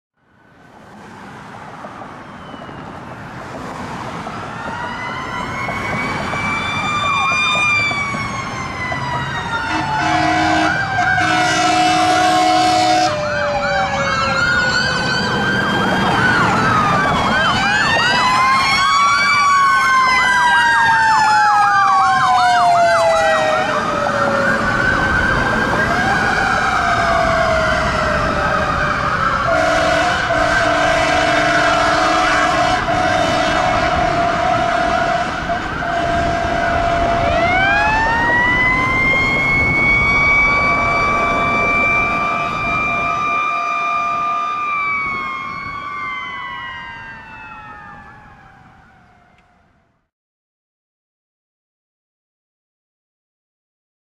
(New) Ladder 23 Responding (Q & Horns)[Cypress Creek Fire Department]
Cypress Creek's new Ladder 23 responding to a medical emergency in their brand new ladder that went in service in early October 2023. It replaced their 2005 Pierce Dash tower truck. Ladder 23 runs as a first out quint company.
Call:
Medical Call

Apparatus:
Ladder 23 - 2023 Pierce Velocity (107' Ascendant/Rear-Mount/Stick)